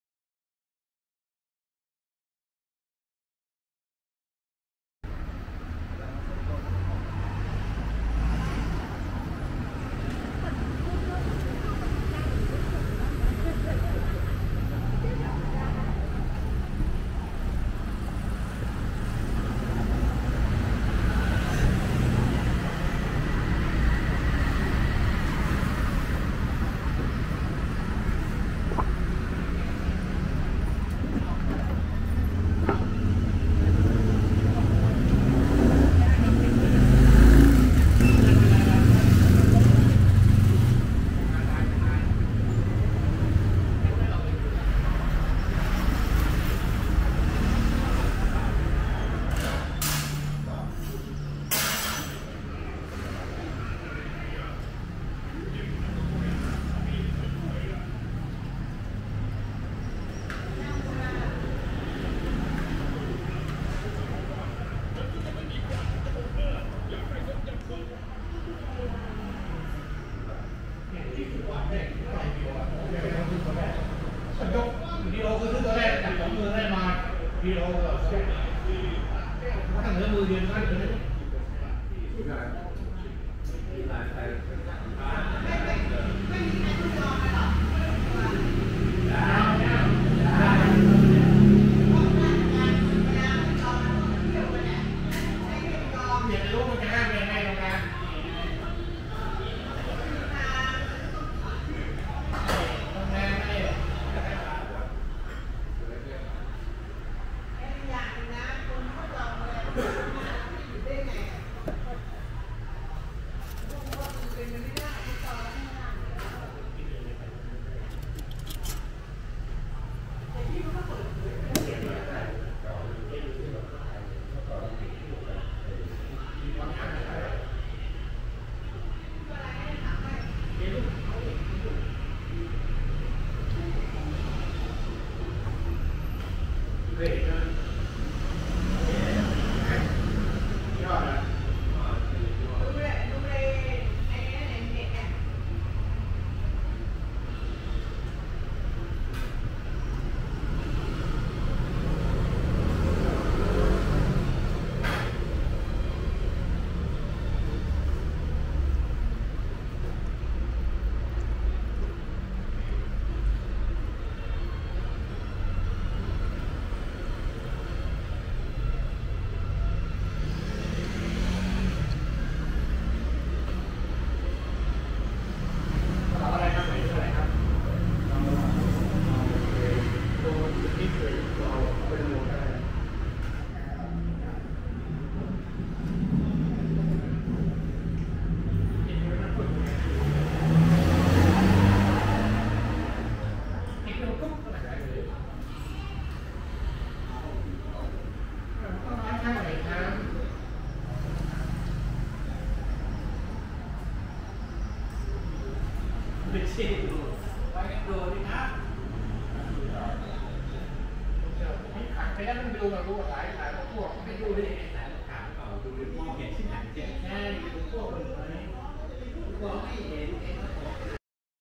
Rote Yiam Beef Noodle เนื้อวัวรสเยี่ยม (แสงตะวัน), Chiang Mai: Michelin Bib Gourmand, 🇹🇭 (Dec'22)
I went to another Michelin Bib Gourmand food stall in Chiang Mai City, Thailand.

This small shop of 30 years has a few different options for beef noodles.

I ordered the mixed beef noodles, which comes with sliced beef, braised beef, beef balls, and beef tendon.

The noodles is very tasty. I will recommend you to try, if a) you are a meat lover, b) you like soup, and c) you are into beef organ meat!

Let's take a bite together!